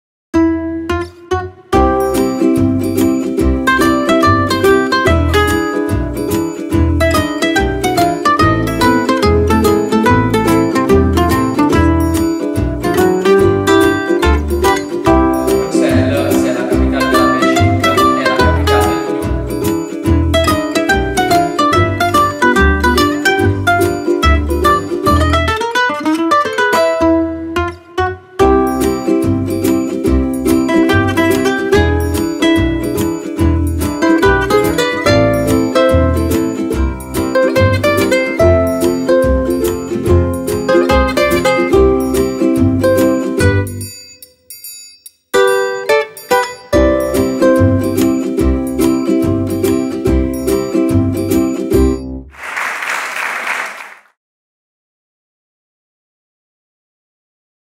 Un approccio metodologico dinamico e interattivo per la lingua francese: la Classe Inversée!
Francophonie: la Belgique. Gli alunni presentano e condividono il risultato del proprio lavoro individuale.

Il video è stato prodotto dal Prof. Dario Fazio con la seguente attrezzatura:
- Obiettivo Sony FE 35mm F1.4 GM